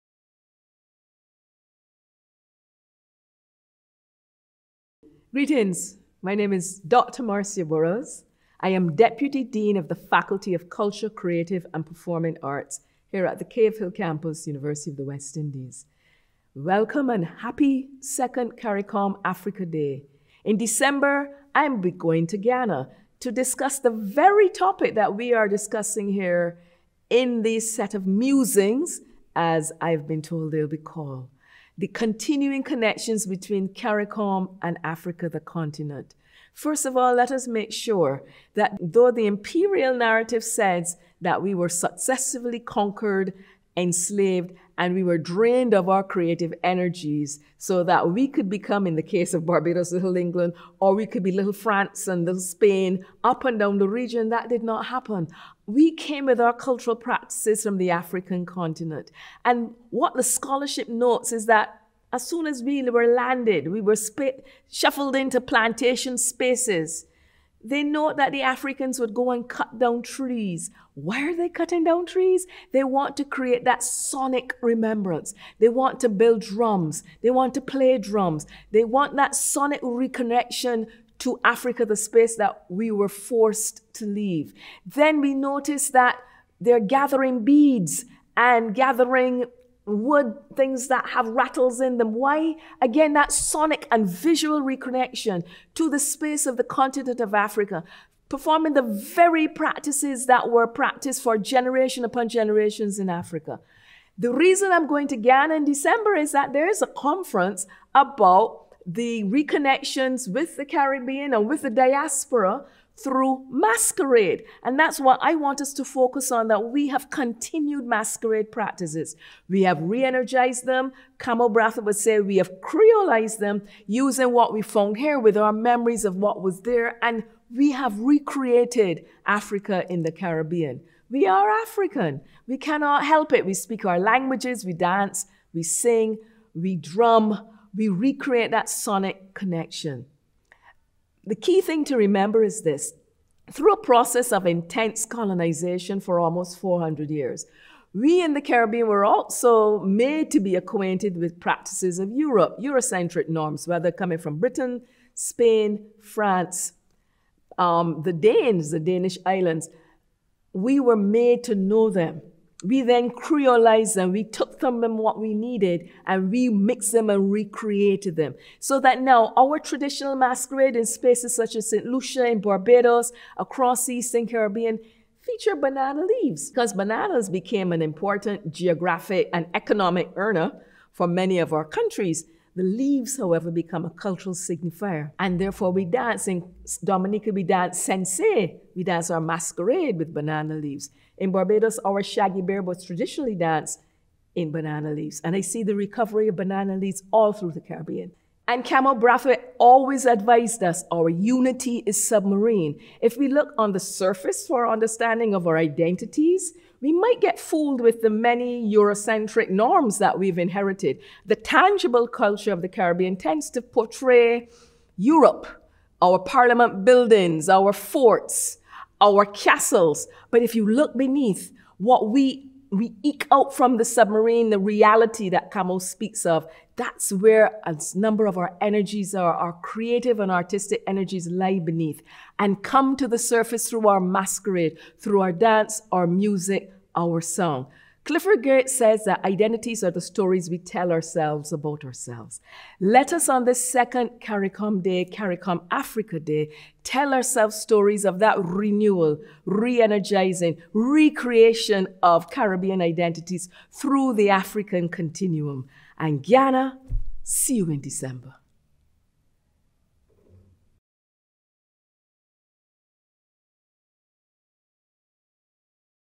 CARICOM-Africa Musings by Dr. Marcia Burrowes
Reflections by Dr. Marcia Burrowes, Deputy Dean in the Faculty of Culture, Creative and Performing Arts at The UWI, Cave Hill.
To celebrate CARICOM-Africa Day 2023, the Office of the Prime Minister has undertaken a number of activities inclusive of CARICOM-Africa Musings, Did You Know posts, and a message to schools and churches. Musings invites academicians, institutional heads and thought leaders to reflect on various aspects of the CARICOM-Africa relationship over time, and their vision for its future development.